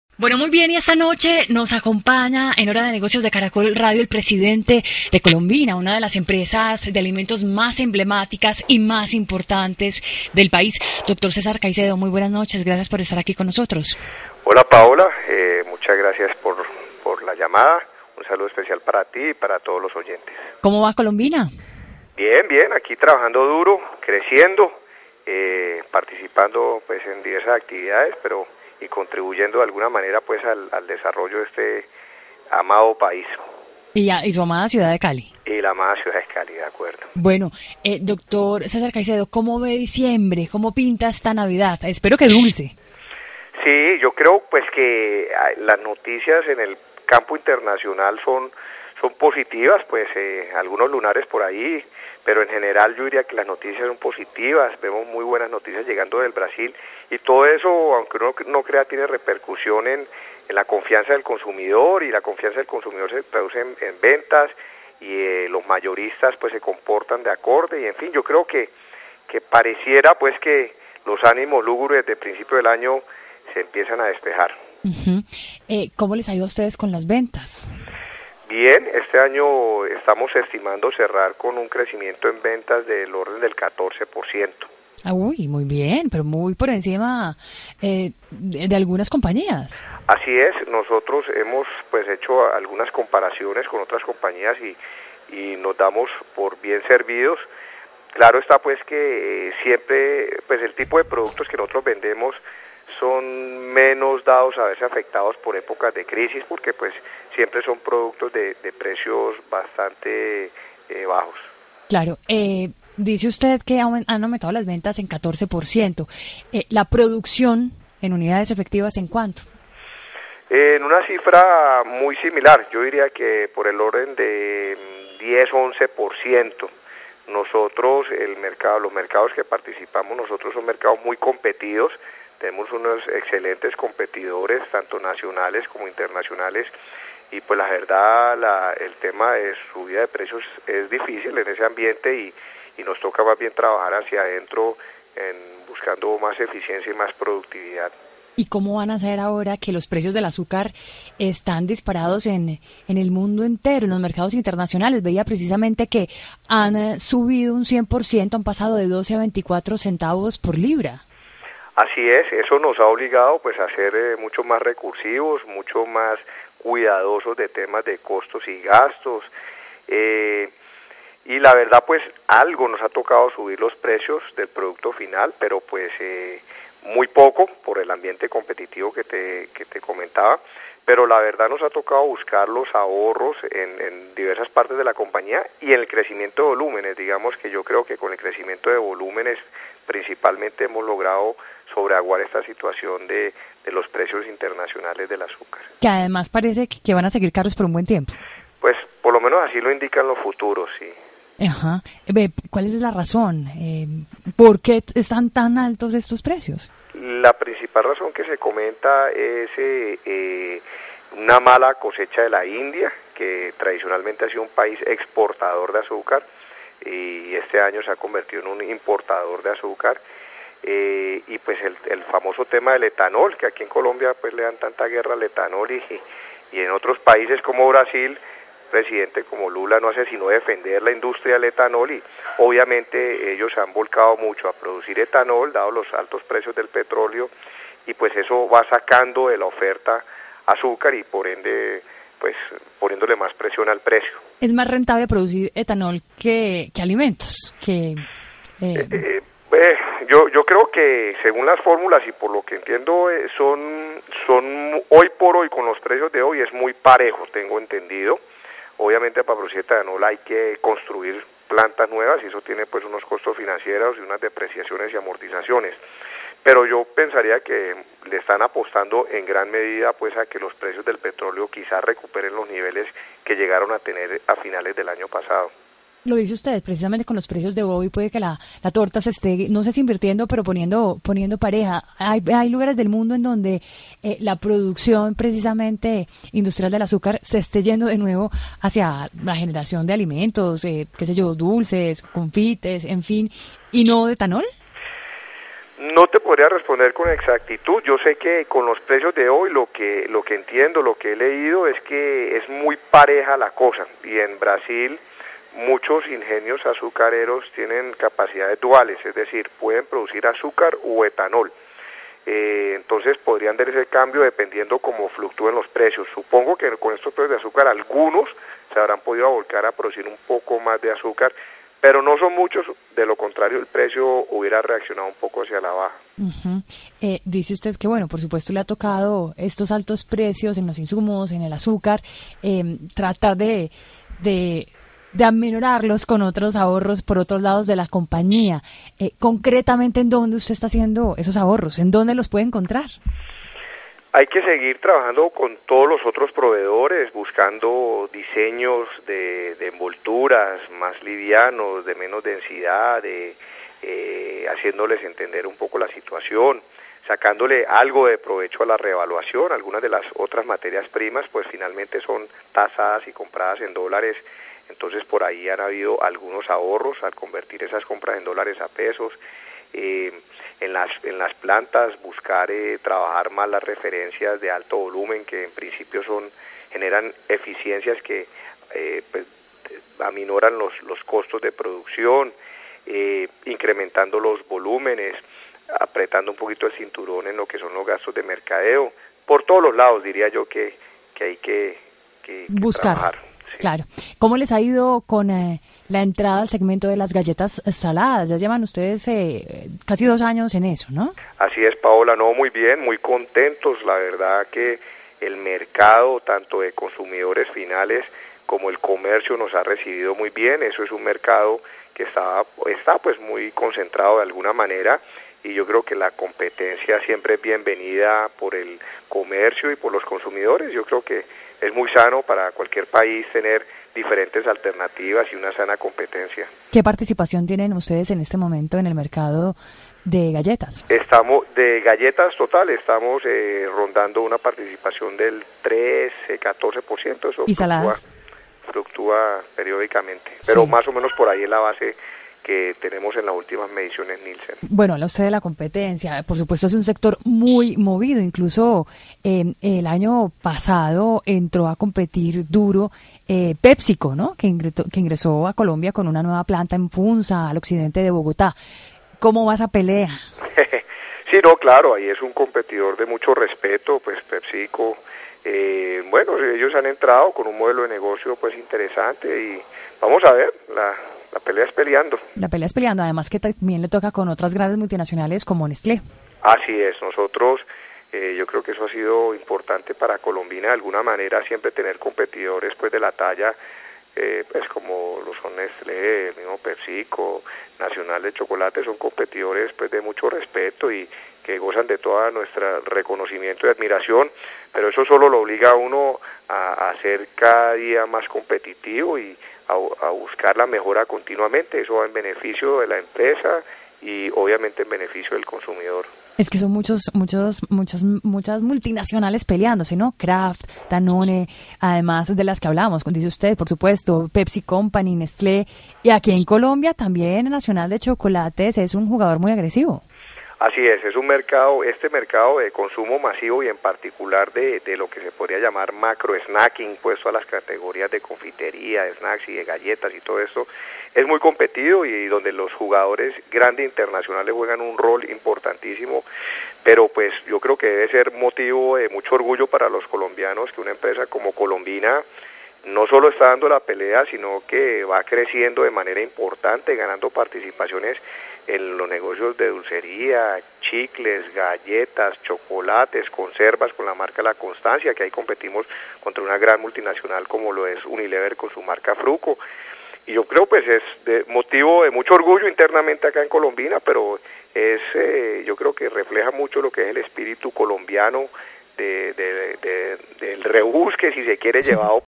Entrevistas empresariales de paola ochoa en La Republica
Entrevistas empresariales de paola ochoa en La Republica - César Caicedo - Presidente Colombina